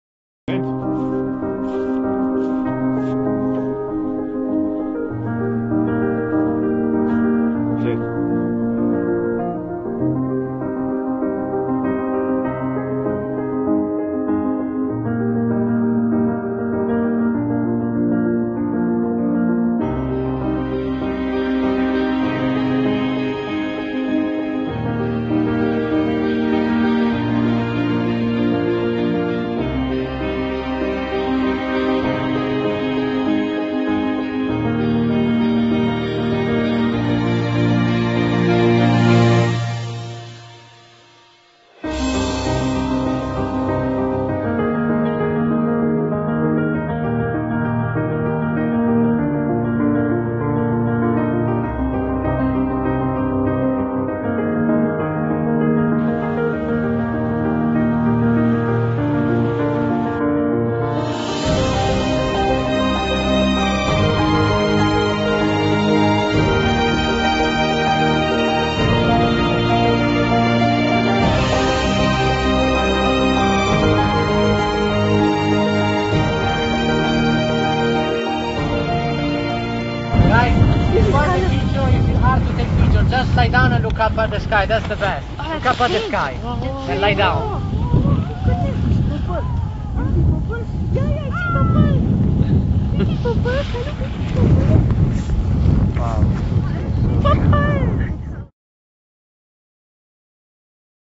Aurora - Amazing View
The Royal Astronomical Society of Canada has designated five places in Alberta as Dark Sky Preserves: Cypress Hills in southeastern Alberta; Jasper National Park; Beaver Hills, east of Edmonton; Lakeland Provincial Park, east of Lac La Biche; and Wood Buffalo National Park in northern Albert